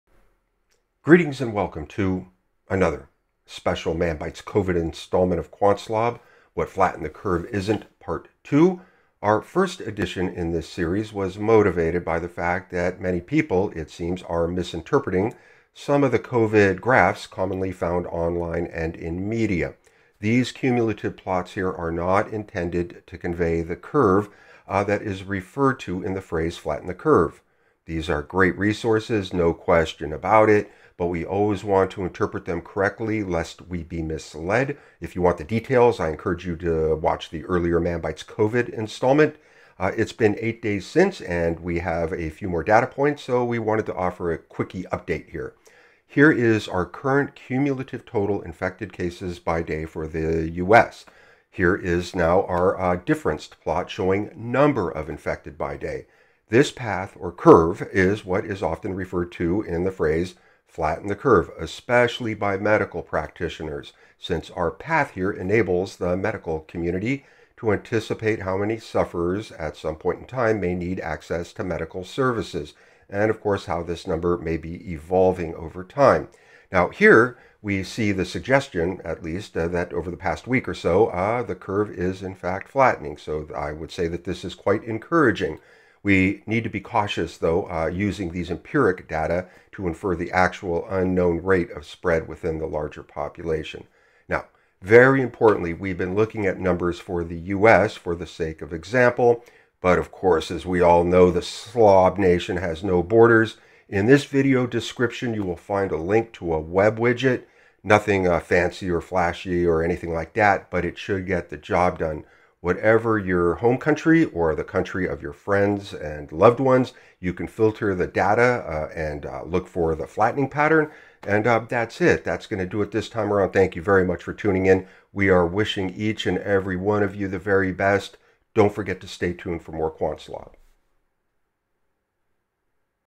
What Flatten The Curve Isn't 2 -- Man Bites COVID Feature 2020-04-19 -- Statistics & Data Science
Quickie COVID-19 update.